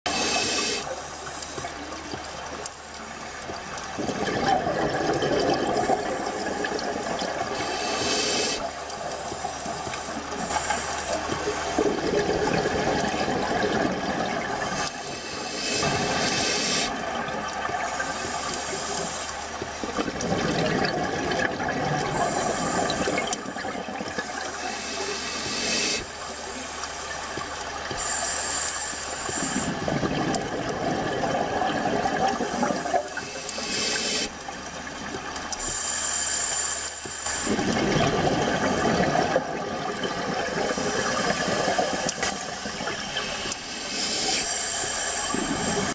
Adventurously Caressing Giant Moray Eel - Moray Eel Sea Monster
Moray eels usually have a larger body size than other sea eels, in particular, some have a length of more than 3m, weighing up to 25kg. Because of this, it is often confused with sea snakes.

They often live hiding in the crevices of rocks and coral during the day to hide from predators and often hunt at night. With sharp teeth and powerful jaw muscles, they are considered one of the most respected hunters of the ocean.

Moray eel - The terrifying obsession of divers! - Photo 1.
Not only that, Moray's teeth have a lot of bacteria, if bitten by it, the victim will be at great risk of infection, gradually leading to necrosis.

Normally, Moray eels are quite shy, so they often hide in coral reefs, only fiercely attacking when they feel threatened or when... hungry! But because they are so shy, they often have a sense of danger when meeting divers or researchers. From there, many scary stories about this sea monster were born.

The content in the video is not mine, but I am summed up from different videos on youtube.